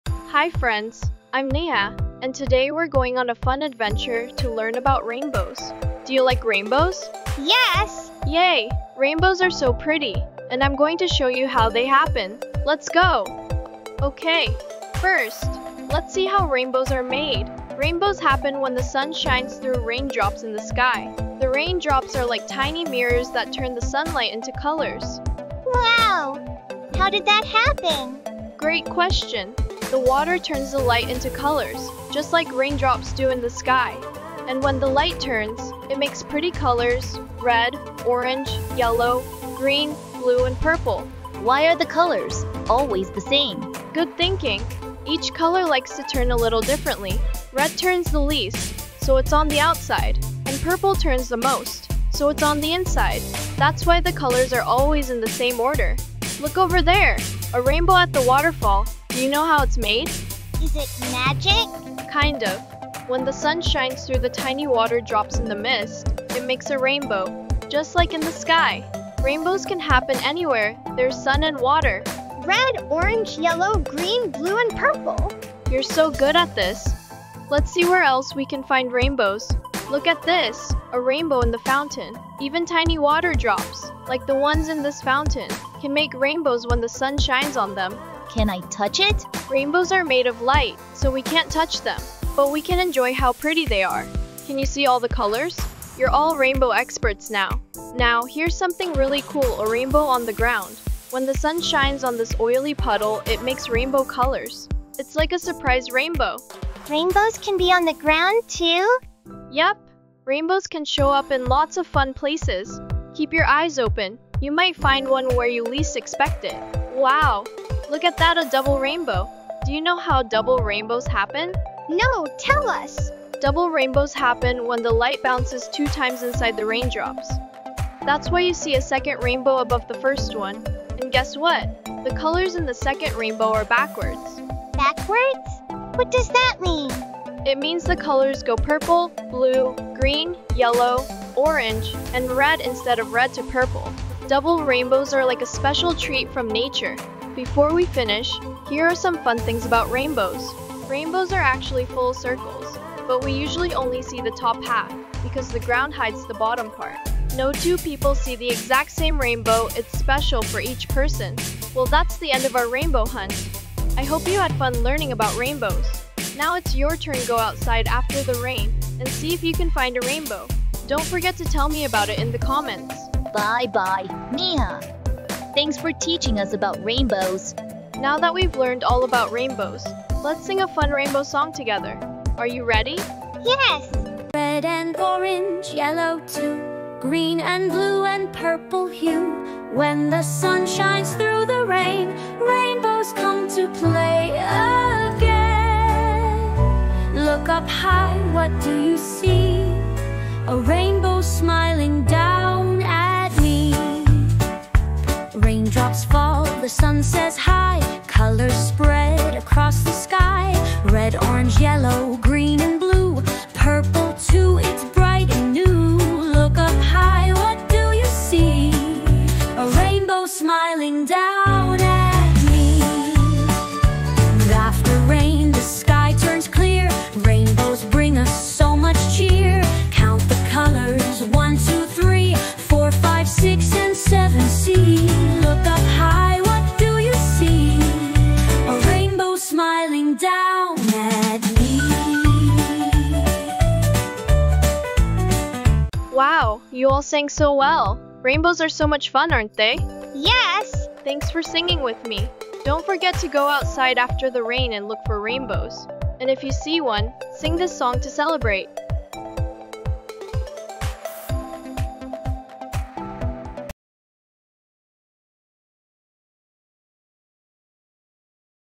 🌈 How Are Rainbows Made? Fun & Easy Science for Kids! 🌦✨
Have you ever wondered how rainbows appear in the sky? 🌈 Join Niha and her friends on a magical adventure to learn all about rainbows! From raindrops to waterfalls, fountains, and even oil slicks—discover the science behind these colorful wonders in a fun and simple way.
💡 Perfect for: Curious kids, parents, teachers, and anyone who loves rainbows!